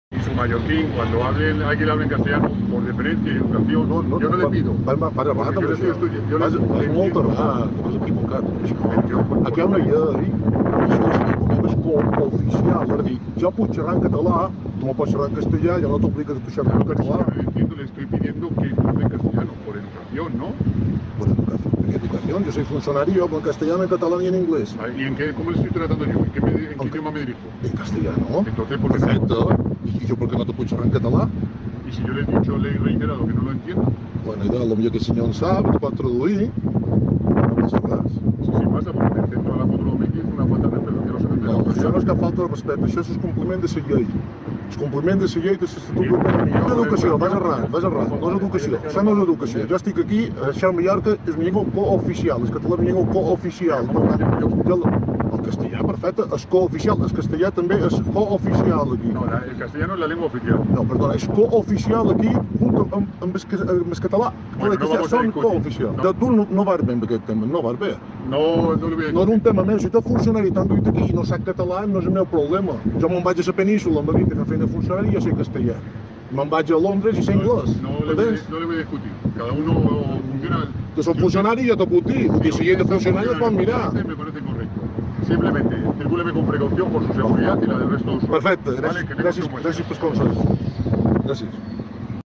[ÀUDIO] Un guàrdia civil a Mallorca exigeix a un motorista que li parli en castellà per “educació”
Plataforma per la Llengua publica un àudio que evidencia que aquest estiu un guàrdia civil va intentar imposar el castellà a un motorista a la carretera d'Artà a Capdepera i va amenaçar de multar-lo per haver-li "faltat al respecte" parlant-li en català.

L'entitat estudia les accions legals que cal emprendre per a perseguir l'actitud de l'agent i per a defensar els drets lingüístics del ciutadà.